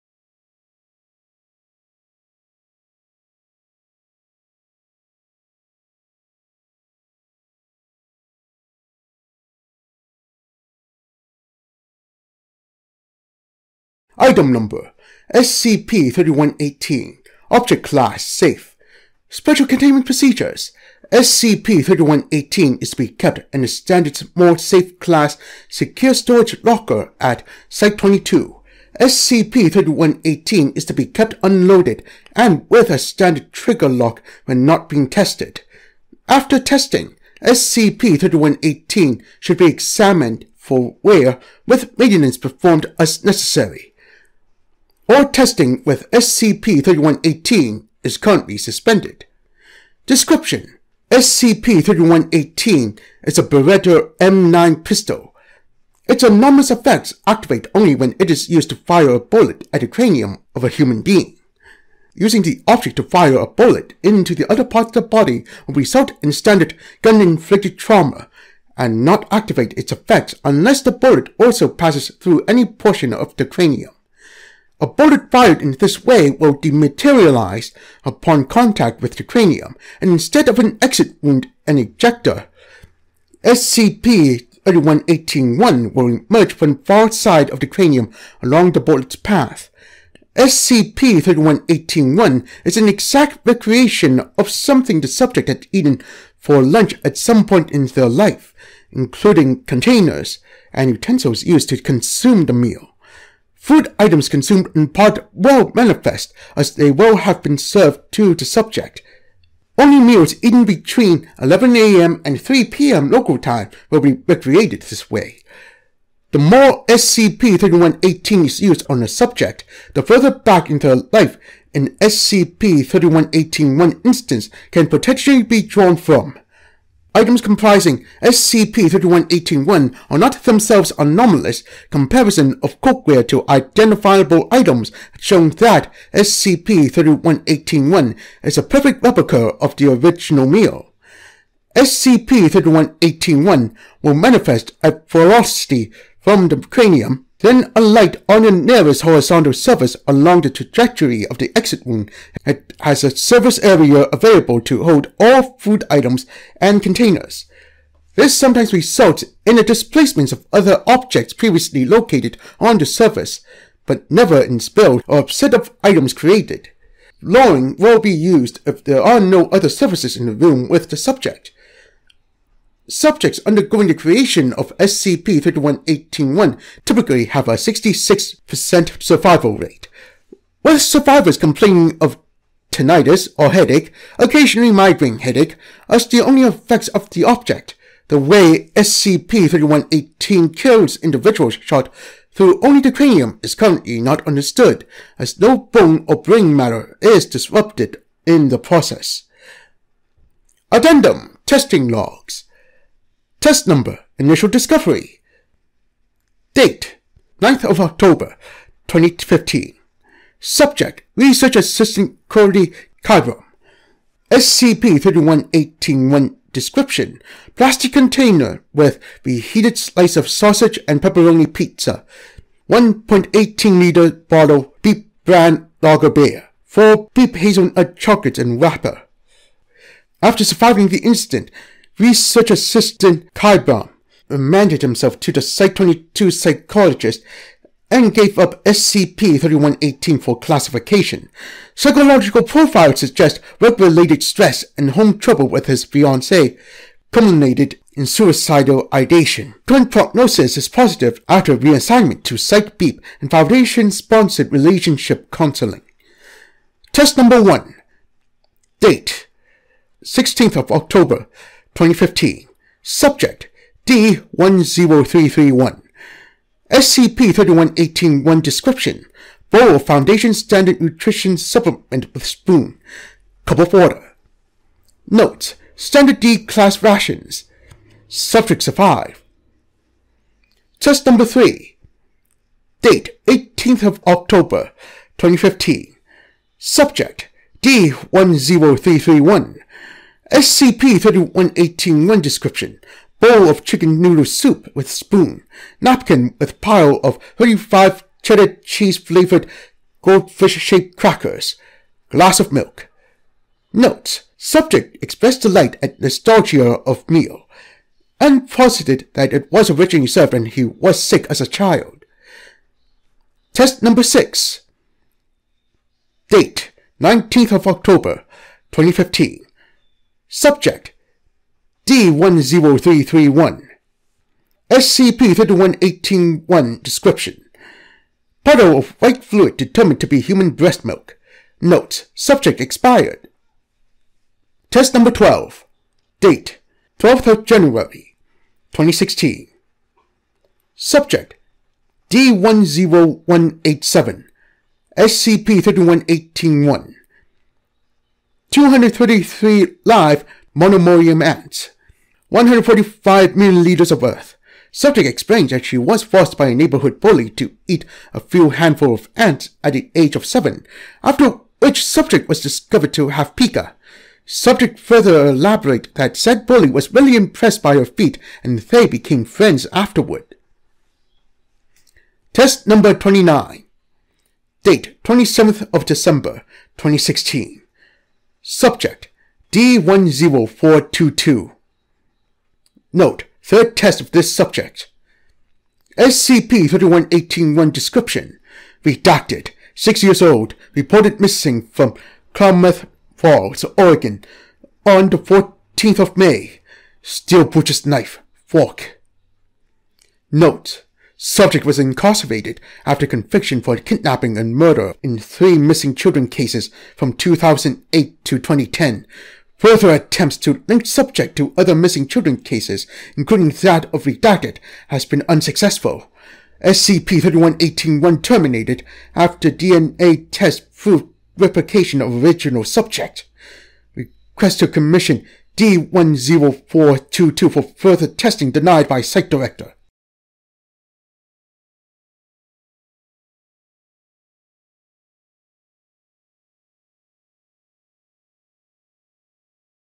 SCP 3118 Lunch Gun
SCP-3118 is a Beretta M9 pistol, when used to shoot someone in the head, while despite having a 33% mortality rate, reproduces said person's lunch from their past.